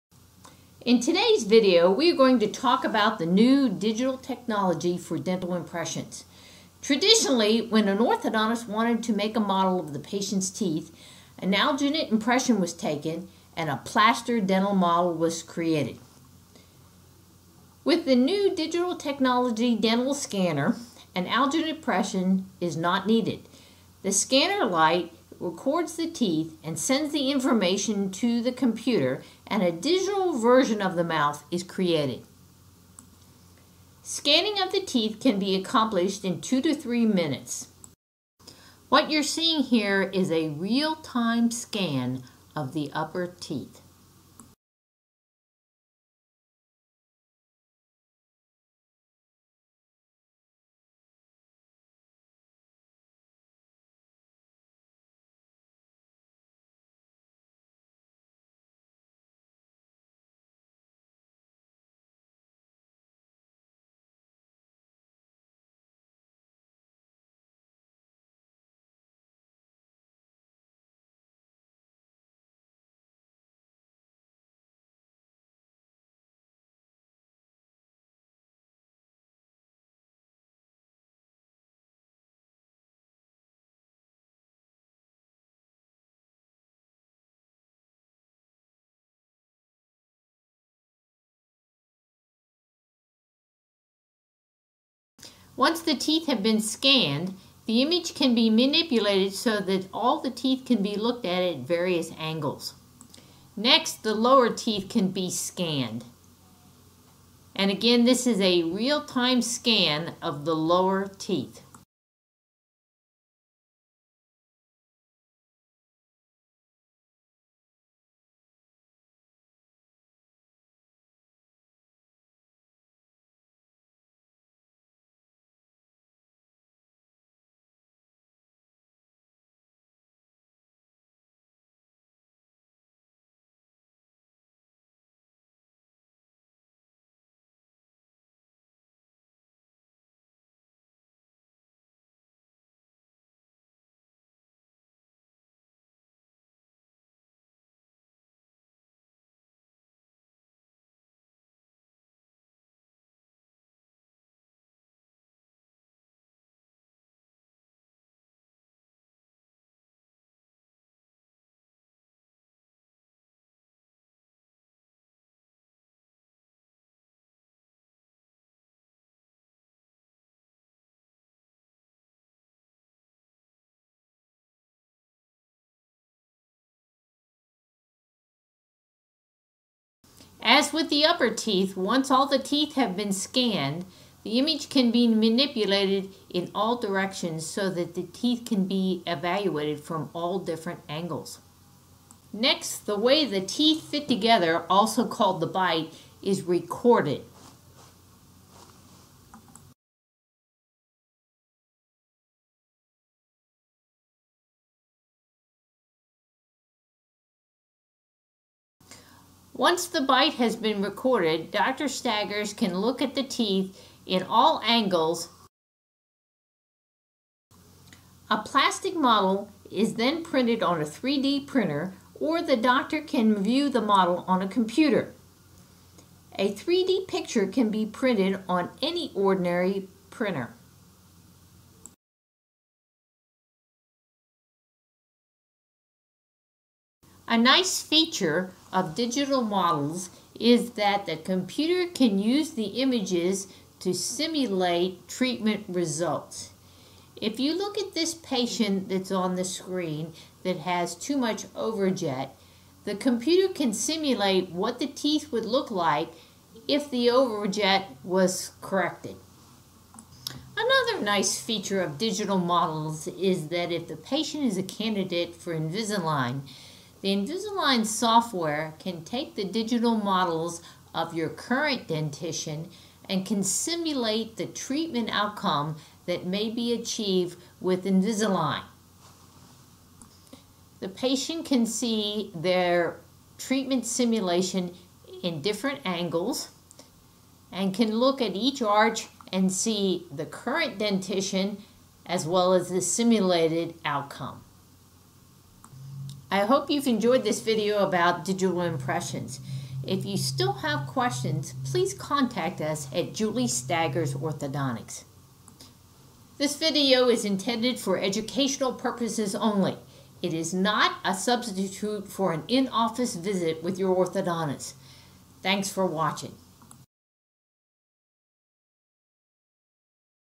Digital Impressions
This video demonstrates the ITero dental scanner for digital impressions.